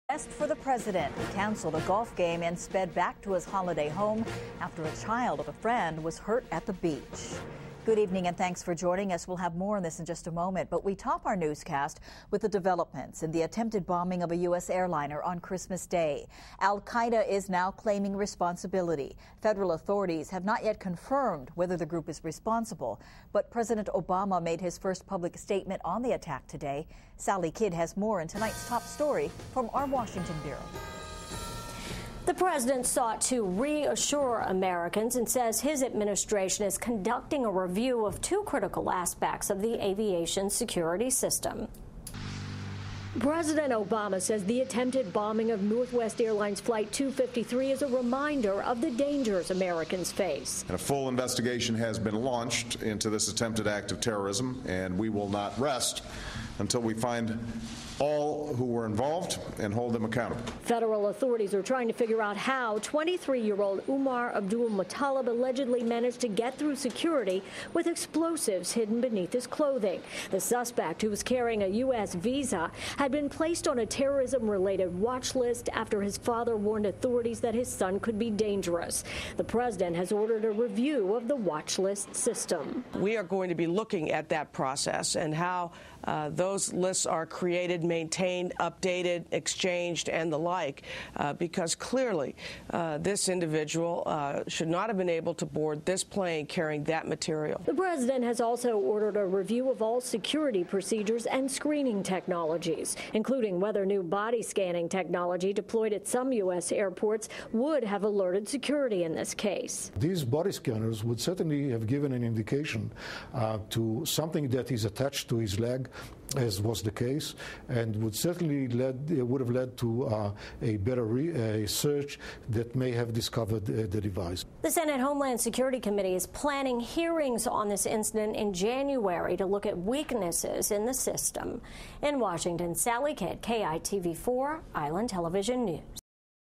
President Addresses Terror Issue From Kaneohe
President Obama made a speech regarding the recent "terror" incidents from Marine Corps Base Hawaii in Kaneohe. 12/28/2009. For news you need to know,